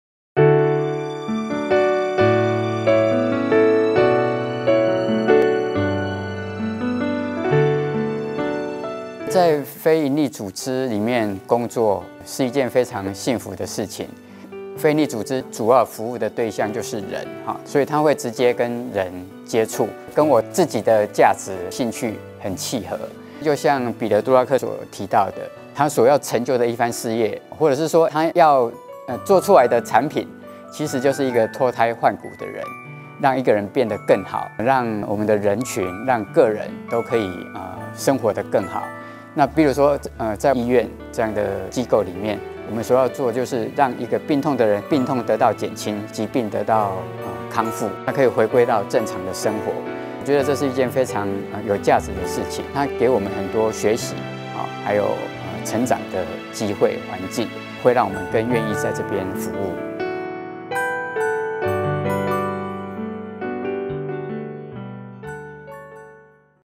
國際服務產業管理學士學位學程—— 第一線與人互動的幸福
慈濟大學國際暨跨領域學院，因應國際服務產業管理人才的需求趨勢，針對NGO暨長照管理與服務管理，全新推出「國際服務產業管理學士學位學程」，於2020年開始招生，歡迎有興趣的高中畢業生踴躍報名，畢業後到全世界各地運用管理，為人服務！

本學程除重視第二外語及服務業管理等專業外，更強調人文禮儀與媒體資訊之素養。藉由紮實的課程培育與多元豐富的業師傳習，建立學生「國際視野、正直領導、問題解決」之核心能力。在學期間安排為期一年之實習以落實學用合一，實習場域含括慈濟海內外「慈善、醫療、教育、人文」四大志業與全球認同慈濟理念之服務與長照產業。

受訪者：門諾基金會總監周弘恬博士

影片製作：慈大媒體製作暨教學中心